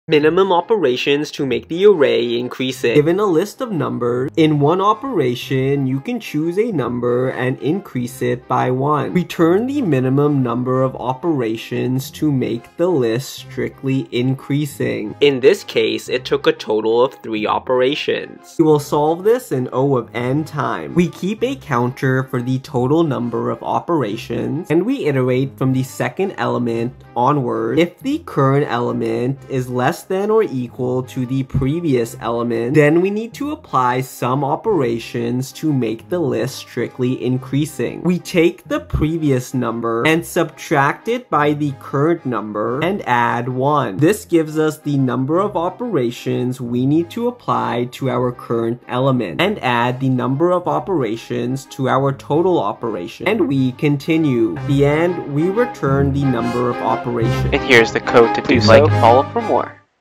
LeetCode#1827 Minimum Operations to Make the Array Increasing - Python
Solving LeetCode problem #1827 Minimum Operations to Make the Array Increasing in Python